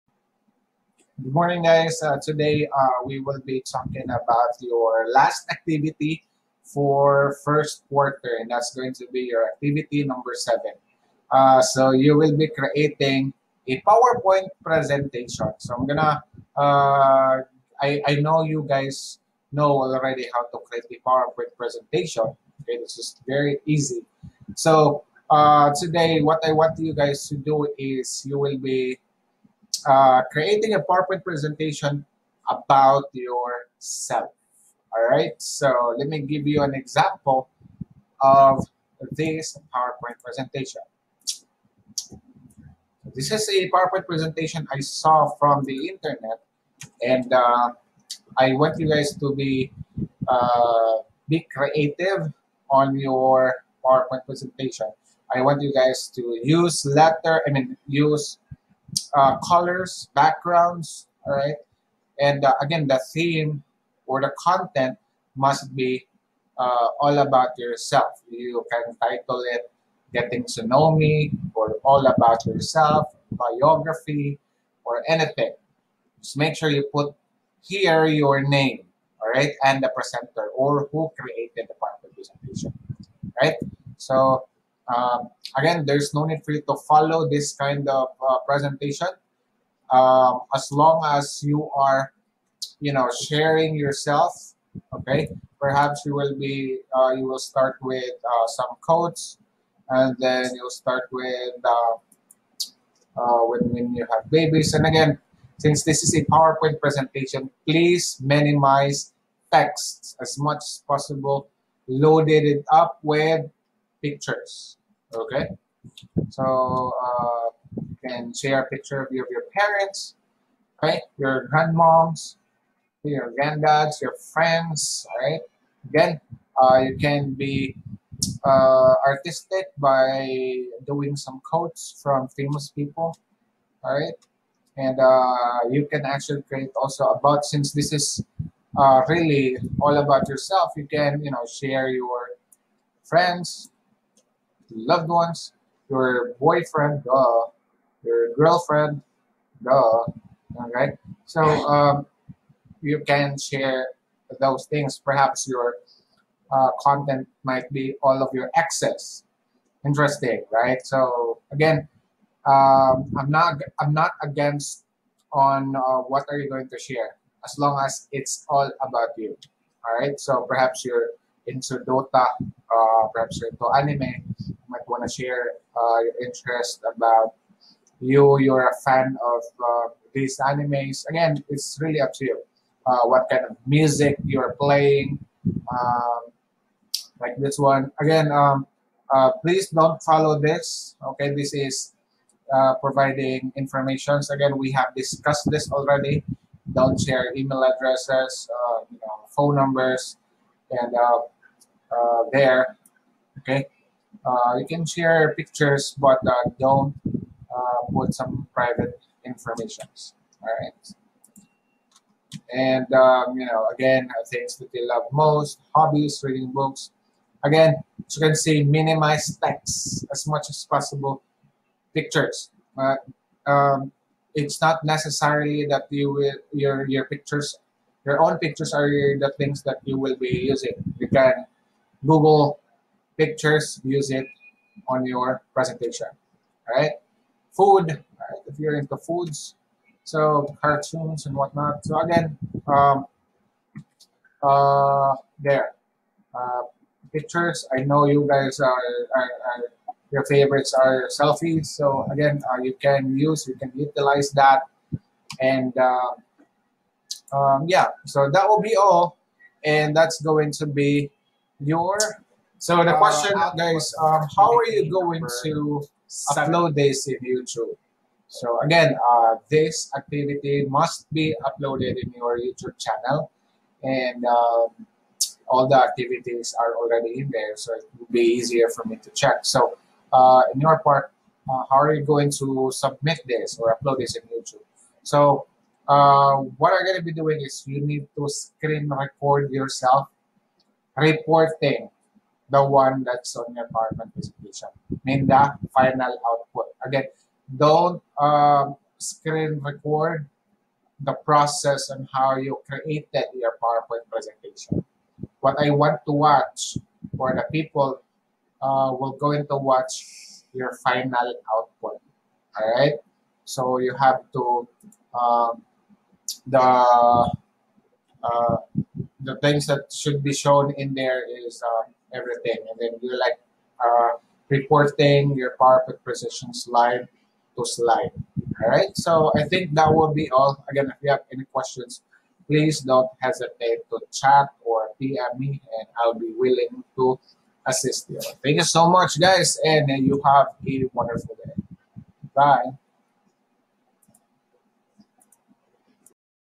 Activity 7: PowerPoint Activity | Empowerment Technologies
This is your 7th and last activity for 1st quarter.
You will create a PowerPoint Presentation about yourself, a biography, or all about you slideshow and you will record yourself reporting your slides via screen recording. Upload it in YouTube once done. The video must be more than 2 minutes including your report.

Note: You will record the reporting of the finish output not the process of creation your report.